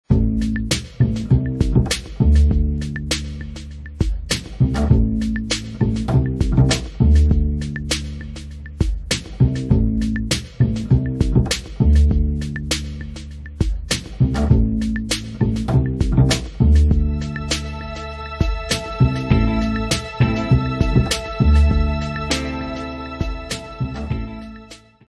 Short video presentation for Barrier Company
Barrier Company installs, manufactures, maintains and repairs a range of security fencing, barriers and acoustic fencing and barriers across the UK.